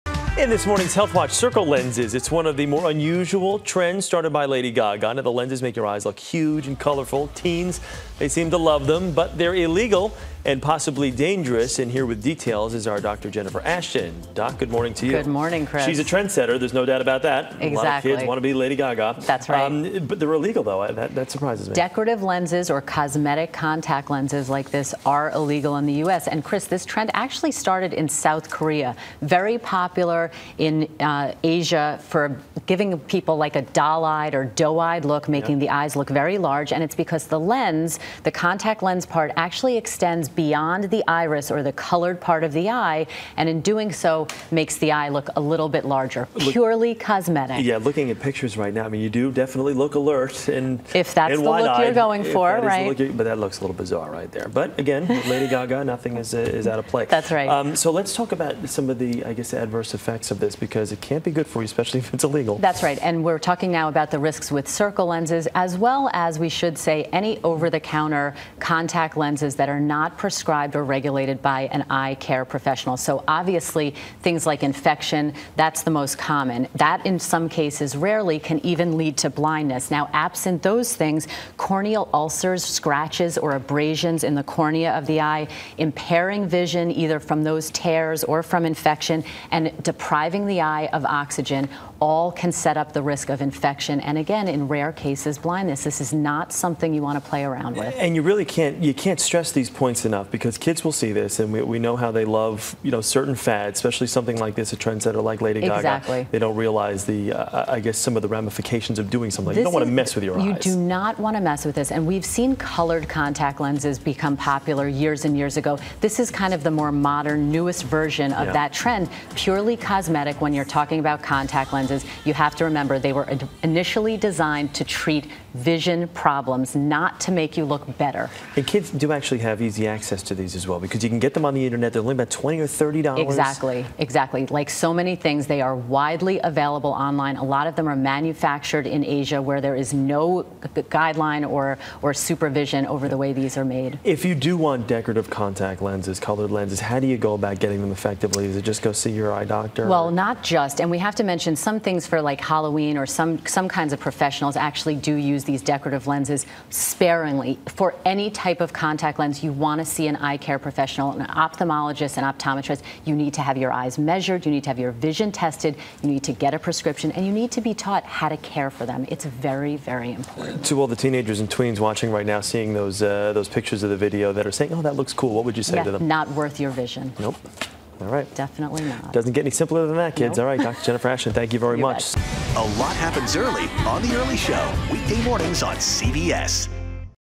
Gaga Contact Lens Controversy
Dr. Jennifer Ashton has the latest on a fad popularized by Lady Gaga that could cause eye problems.